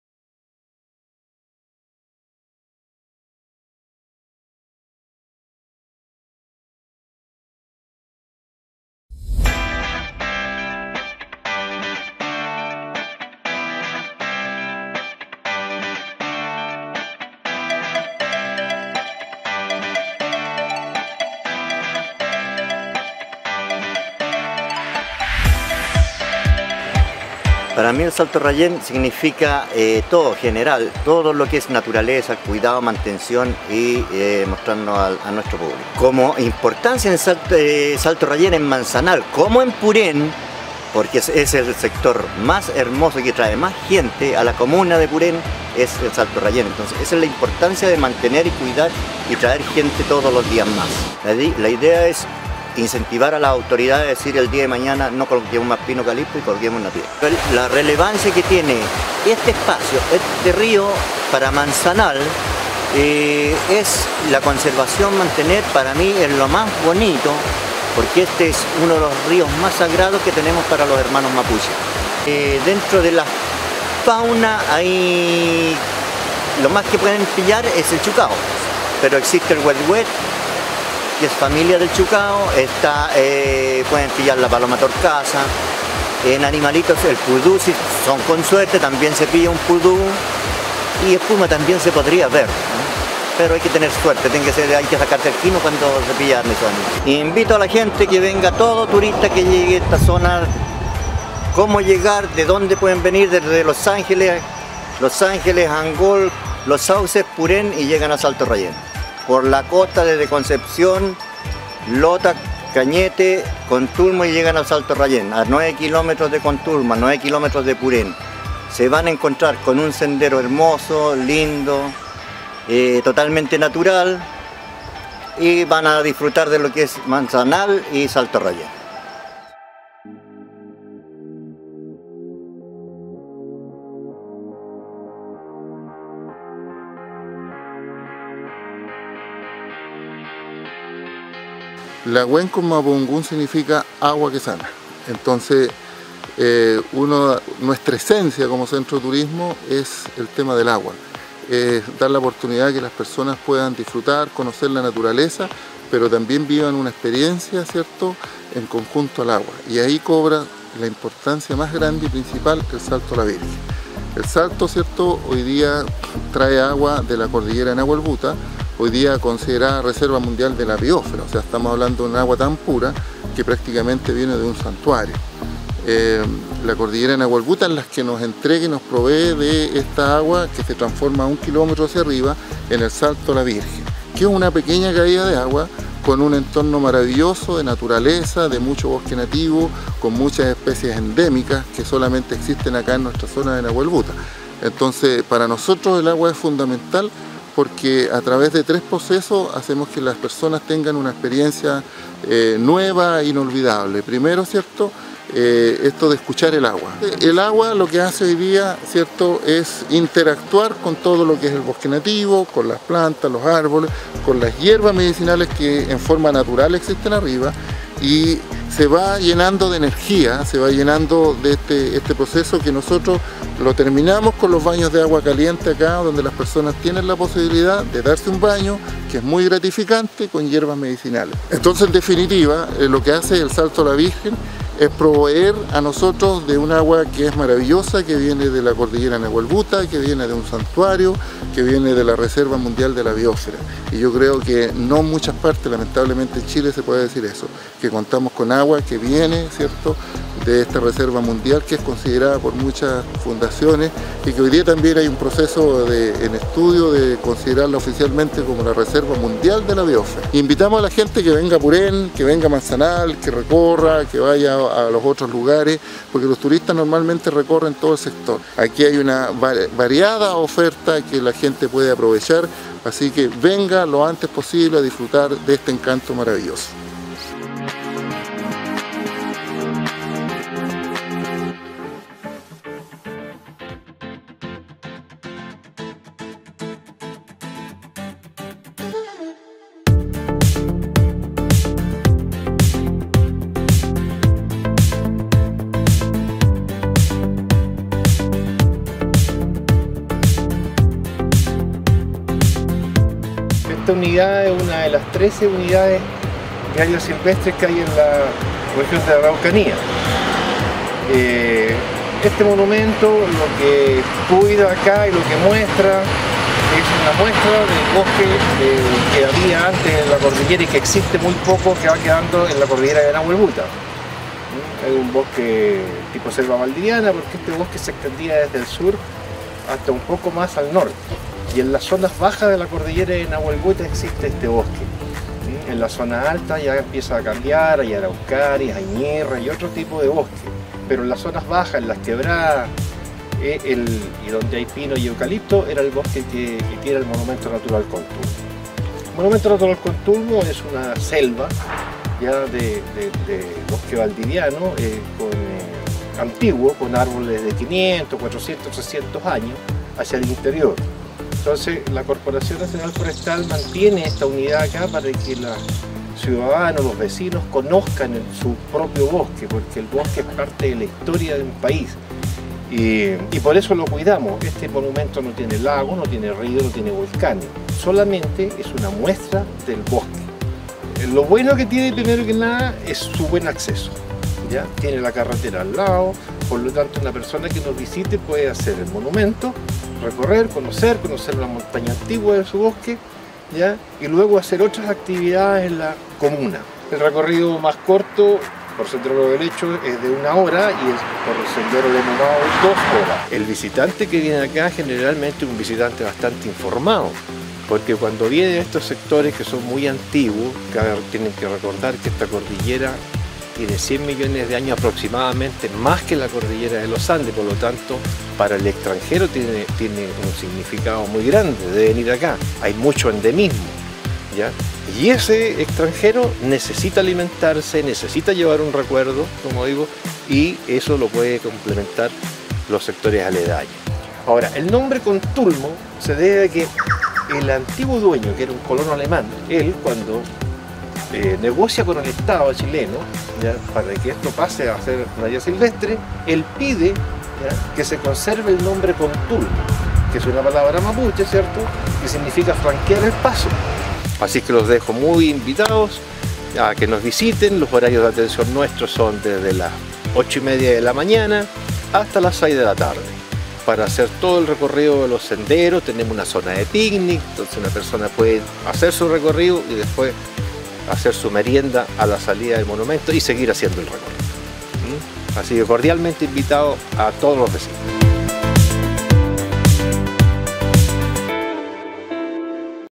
Atractivos Turísticos, Manzanal – Microdocumentales Servicio País Purén
La Cordillera de Nahuelbuta alberga en sus bosques variedad de flora y fauna endémica, además de ofrecernos hermosas vistas y parajes maravillosos.

Trabajo realizado por el equipo de Servicio País Araucanía, 2019 y el equipo del Área de Comunicaciones de la Municipalidad de Purén.

Purén, Región de La Araucanía, Chile, 2019